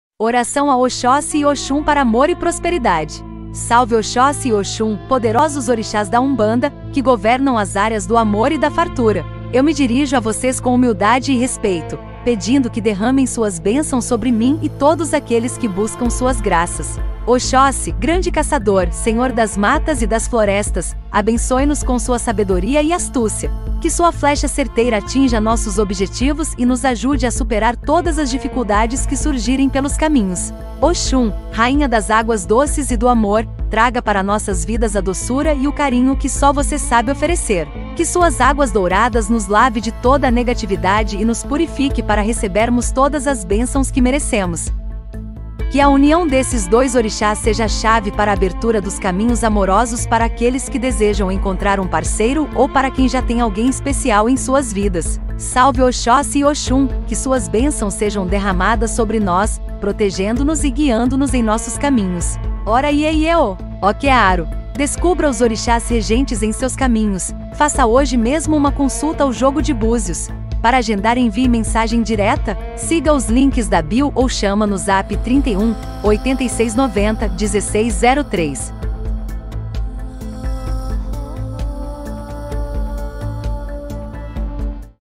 Oração a Oxóssi e Oxum para amor e prosperidade
🏹💚💛Deixe seu "Ora yê Yê ô e Okê Arô!" nos comentários. 🙏

"💚💛Salve Oxóssi e Oxum, poderosos orixás da Umbanda, que governam as áreas do amor e da fartura. Eu me dirijo a vocês com humildade e respeito, pedindo que derramem suas bênçãos sobre mim e todos aqueles que buscam suas graças.

💚🌳Oxóssi, grande caçador, senhor das matas e das florestas, abençoe-nos com sua sabedoria e astúcia.

🏹Que sua flecha certeira atinja nossos objetivos e nos ajude a superar todas as dificuldades que surgirem pelos caminhos.

💛💐Oxum, rainha das águas doces e do amor, traga para nossas vidas a doçura e o carinho que só você sabe oferecer.

💦😍Que suas águas douradas nos lave de toda a negatividade e nos purifique para recebermos todas as bênçãos que merecemos.

💛💚Que a união desses dois orixás seja a chave para a abertura dos caminhos amorosos para aqueles que desejam encontrar um parceiro ou para quem já têm alguém especial em suas vidas.

🏹💐Salve Oxóssi e Oxum, que suas bênçãos sejam derramadas sobre nós, protegendo-nos e guiando-nos em nossos caminhos. Ora yê yê ô! Okê Arô!" 😍🙏

👉Faça hoje mesmo sua consulta por apenas R$ 60,0 e descubra seus 3 orixás (principal, ajunto e ancestral) .😀🙏 siga o